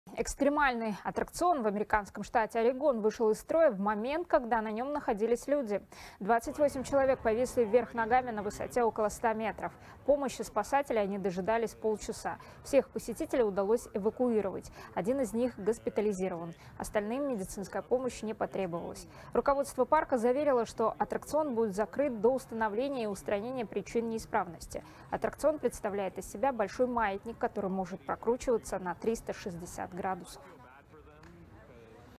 На экстремальном аттракционе в США застряли 28 человек
Экстремальный аттракцион в американском штате Орегон вышел из строя в момент, когда на нём находились люди. 28 человек повисли вверх ногами на высоте около 100 метров, сообщает «24KZ».